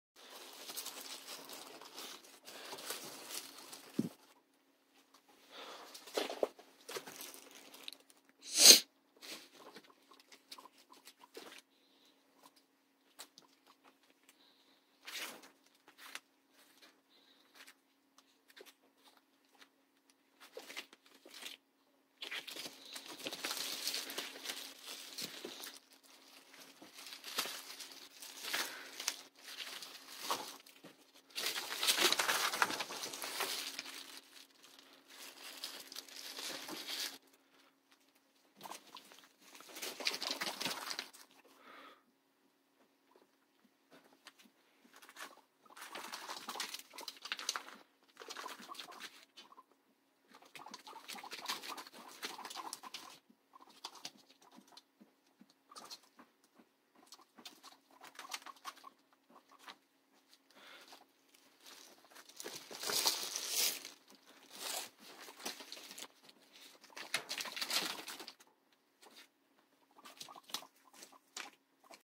Miminie and Peepeeson looking for food when I'm cleaning poop
no food till I'm done cleaning!